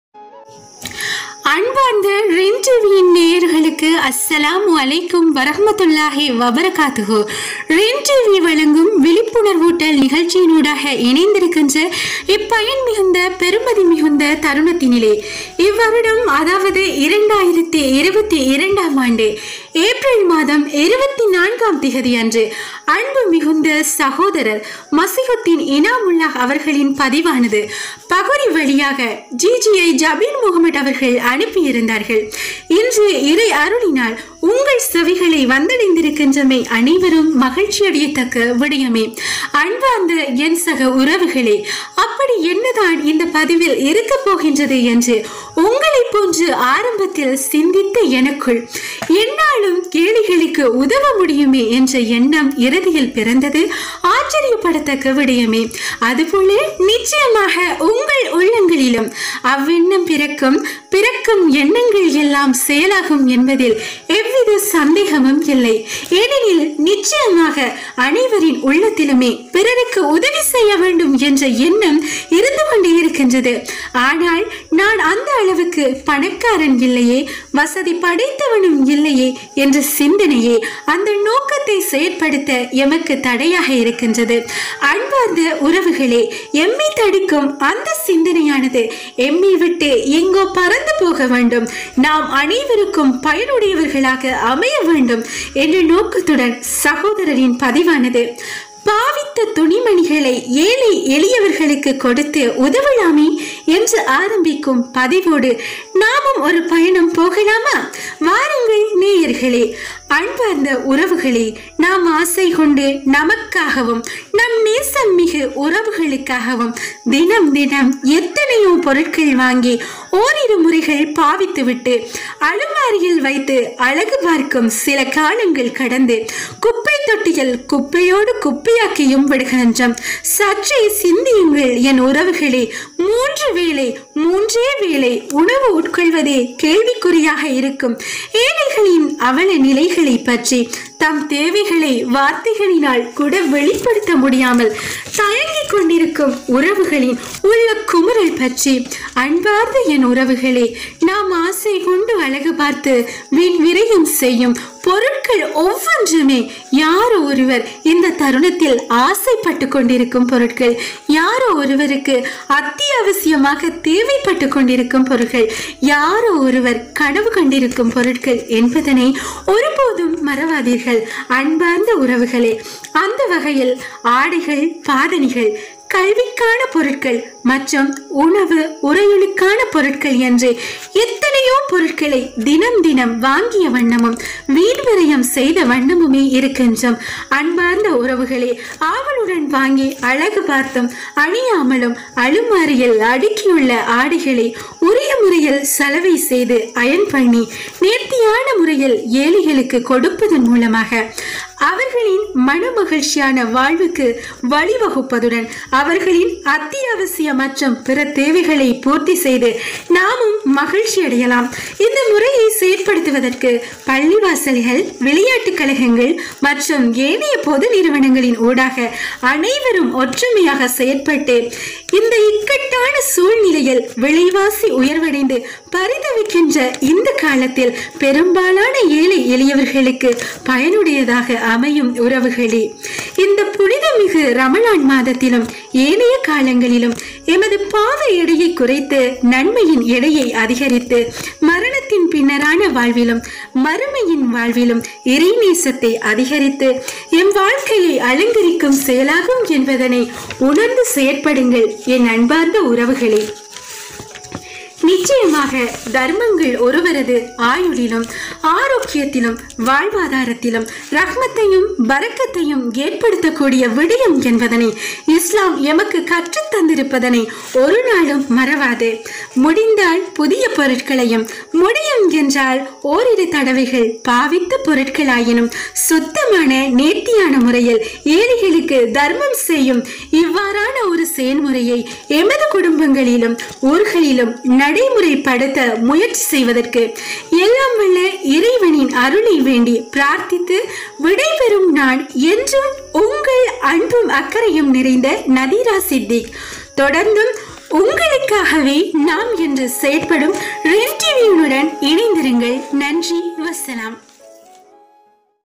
விழிப்பூட்டல் பதிவு. சிந்திக்க சில நொடிகள்
ரின் டிவி யின் விழிப்புணர்வுக்கான பதிவு*

24/04/2022 திகதி
மஸிஹுத்தீன் இனாமுல்லாஹ்
அவர்களின் பதிவை

எமக்கு பகுரி வழியாக GGI JABEEN MOHAMED  அனுப்பி வைத்திருந்தார்

ஆம்  நேயர்களே இந்த பதிவினை சற்று அவதானிப்போம்

பாவித்த துணிமணிகளை ஏழை எளியவர்களுக்கு கொடுத்து உதவலாம்

என்று அவர் இவ்வாறு இந்த பதிவை ஆரம்பித்துள்ளார்

ஆம்
உங்கள் வீடுகளில் ஓரிருமுறைகள் பாவித்து விட்டு  பாவிக்காமல் அடுக்கி வைக்கப் பட்டுள்ள நல்ல நிலையில் உள்ள ஆடைகளை

ஏழை எளியவர்கள்  பெற்றுக் கொடுப்பதற்கான ஏற்பாடுகளை செய்யலாம்

ஆம் நேயர்களே இது ஒரு நல்ல செயல் முறை வசதி

தான தர்மம் செய்வதற்கு வசதி
 படைத்தவர்களாக இருக்க வேண்டிய அவசியம் இல்லை என்ற விடயத்திற்கு முன்னுதாரணமாக இந்தத் திட்டம் அமைகின்றது

 ஏழை எளியவர்களுக்கு இந்த முறையிலும் தர்மங்களை செய்யலாம்

இந்த முறையை செயல் படுத்துவதற்கு
 பள்ளிவாசல் விளையாட்டு கழகங்கள் மற்றும் ஏனைய நிறுவனங்களின் ஊடாக திட்டமிட்டு இதை செயல் படுத்தலாம்

நீங்கள் உங்கள் சிறுவர்களுக்கு வாங்கப்பட்ட புதிய அதிகம் பாவிக்கப்படாத ஆடைகளை

 அவர்கள் வளர்ந்து செல்கின்ற போதிலும் வீடுகளில் பாதுகாப்பாக வைத்திருப்பீர்கள்

அவ்வாறு பாதுகாப்பாக வைக்கப்பட்ட ஆடைகள் மற்றும் தம்மிடம் மேலதிகமாக இருக்கின்ற ஆடைகள் சிலவற்றையும் பலரும் தருவதற்கு முன்வருவார்கள்.

தற்போதைய சூழ்நிலையில் விலைவாசி வாழ்வாதார உயர்வை கருத்தில் கொண்டு

வேறு அத்தியாவசிய தேவைகளுக்காக தனக்கு கிடைக்கக்கூடிய பணத்தை சேமித்துக் கொள்ள விரும்புகின்ற தேவையுடையவர்களும் இந்த ஏற்பாடின் மூலம் பயனடையலாம்!

ஆடைகள் மாத்திரமன்றி நல்ல நிலையில் உள்ள பாதணிகளையும் பாடசாலை உபகரணப் பொருட்கள்  அனைத்தையும் இவ்வாறு வழங்கலாம்.

அவ்வாரு சேகரித்து வழங்கப்படும் பொருட்களை மற்றும் ஆடைகளை  சலவை செய்யது

 உரிய வகையில் அயர்ன் பண்ணப்பட்டு ஏழைகளுக்கு கௌரவமான முறையில் ஒப்படைக்கப் படுவது சிறந்த முறையாகும்,

 அல்லது அவற்றை சேகரித்து வழங்குவதற்கு  ஏற்பாடுகளை செய்கின்றவர் உரிய முறையில் திட்டமிட்டு செயல்படுத்தலாம்

நிச்சயமாக இது ஒரு சிறந்த ஸதகாவாகவும்

 இந்தப் புனித ரமழான் மாதத்திலும் ஏனைய காலங்களிலும் நன்மைகளை ஈட்டித் தரும் தர்மமாகவும் அமைந்து விடும்

நிச்சயமாக தர்மங்கள் ஒருவரது ஆயுலிலும் ஆரோக்கியத்திலும் வாழ்வாதாரத்திலும் ரஹமத் தையும் பரக்கத்தையும் ஏற்படுத்தக் கூடிய விடயம் என்பதனை இஸ்லாம் எமக்கு கற்றுத் தருகின்றது.

ஆம் நேயர்களே இவ்வாறான ஒரு செயல்முறையை எமது ஊர்களிலும் நடைமுறைப்படுத்துவதற்கு நாம முயற்சிப்போம் என்று கூறிக்கொண்டு

 *விடைபெறும் நான் என்றும் போல் உங்கள் அன்புள்ள நதீரா*

நன்றி வஸ்ஸலாம்